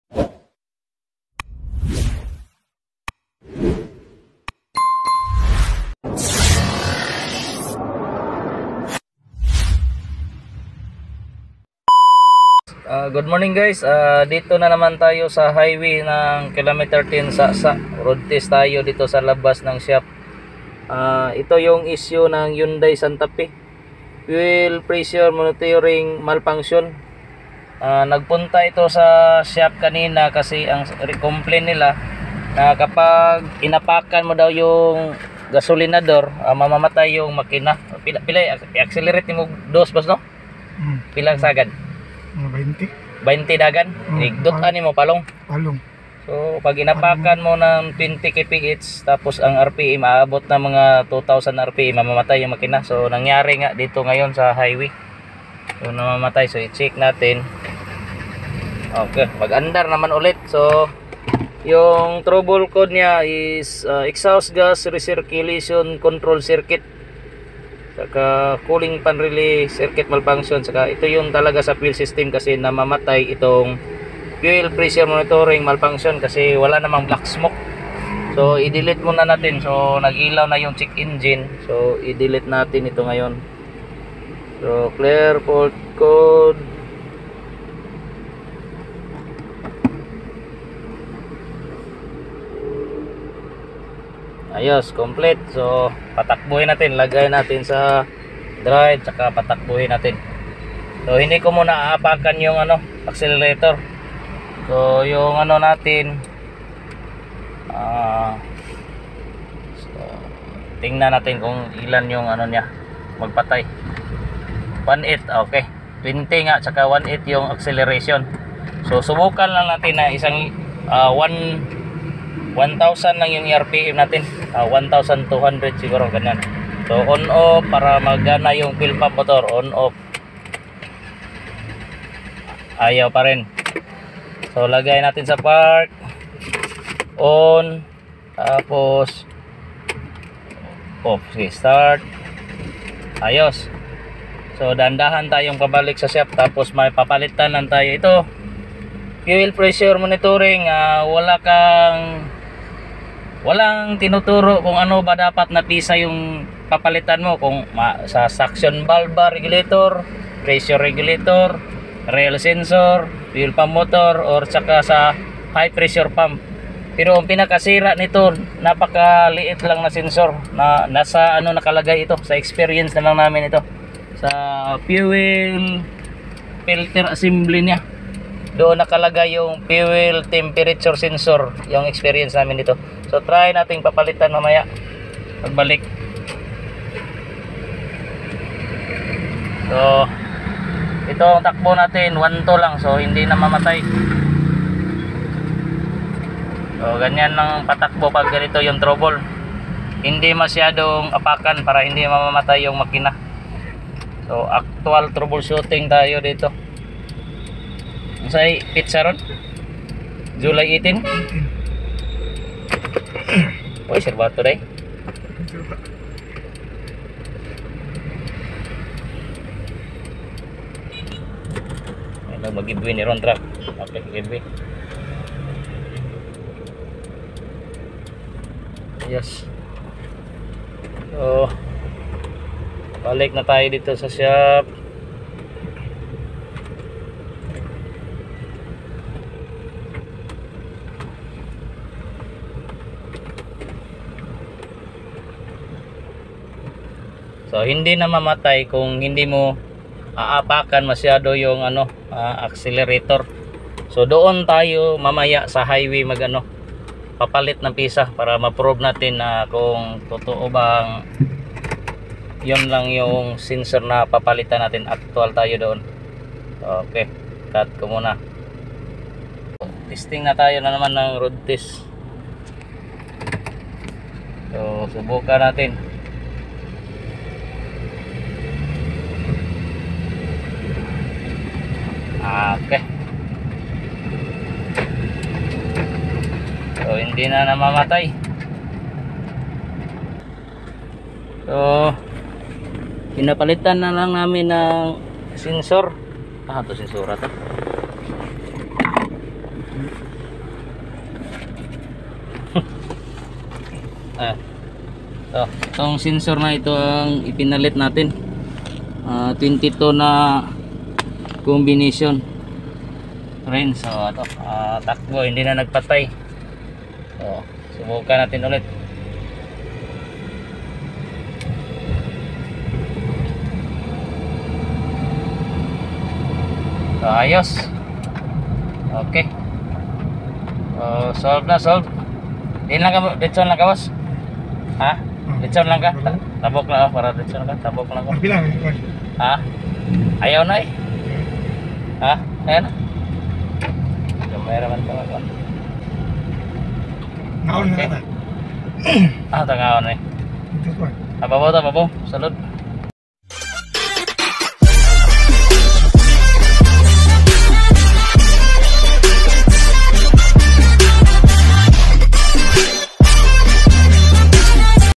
Hyundai Santa Fe 2004 p1181,p1624,p0403.
p1181 fuel pressure monitoring mal function,engine shut while pressing accelerator pedal,engine shut off at 20kph.